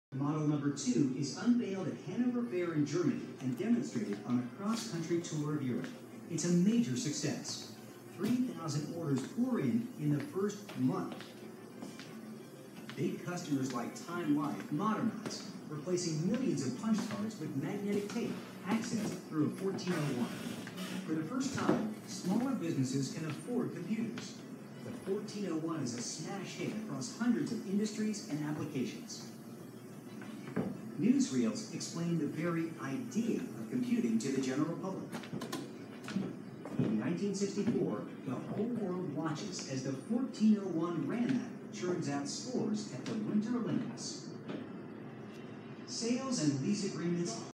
Computer History Museum - The IBM 1401
The IBM 1401 is a variable wordlength decimal computer that was announced by IBM on October 5, 1959. The first member of the highly successful IBM 1400 series, it was aimed at replacing unit record equipment for processing data stored on punched cards and at providing peripheral services for larger computers